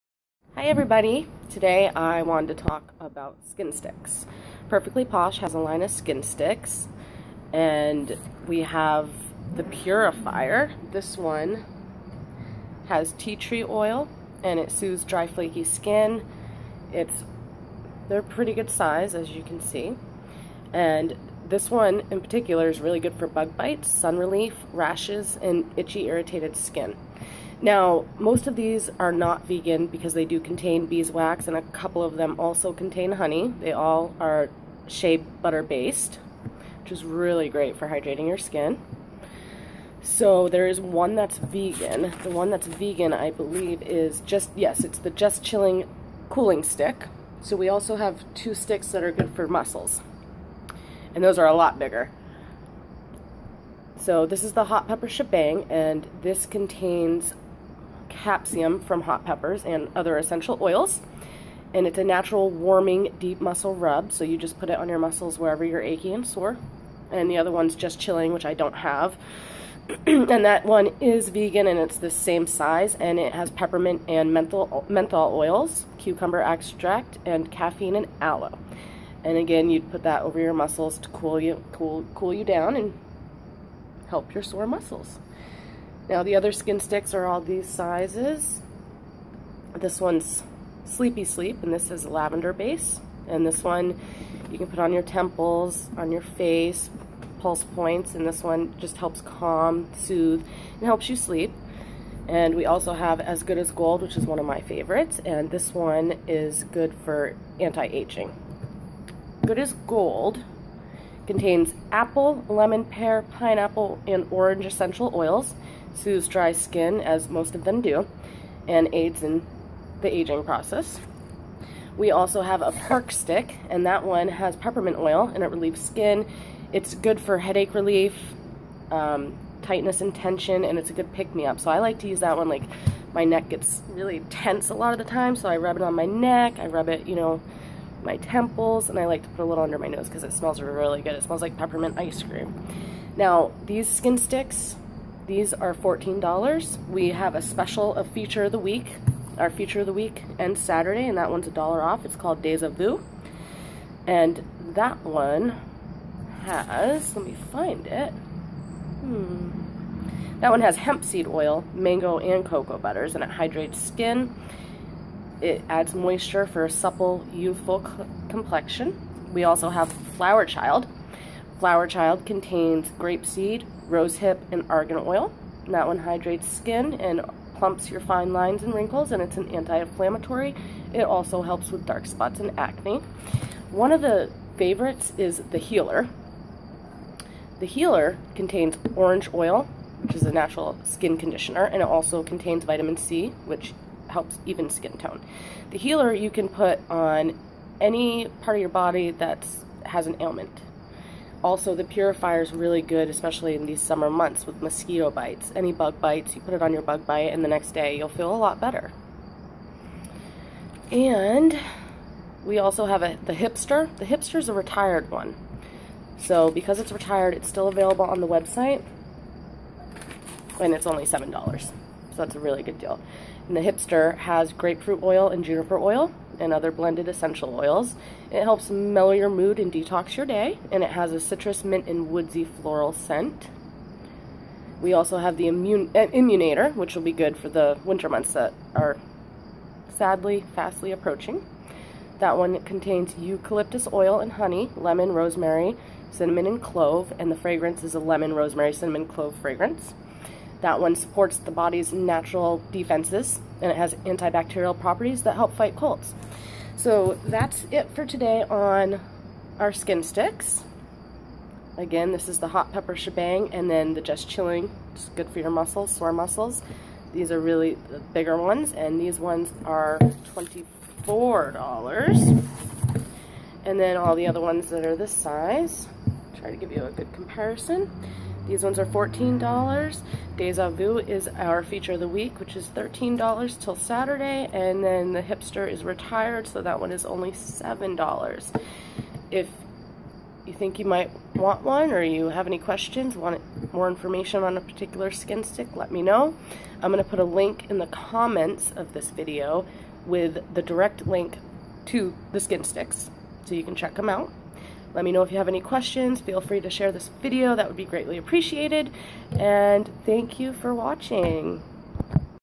Perfectly Posh Skin Sticks
In this video I will be talking about Perfectly Posh's skin stick line. These are excellent for rashes, anti-aging, bug bites, fighting off colds and more!!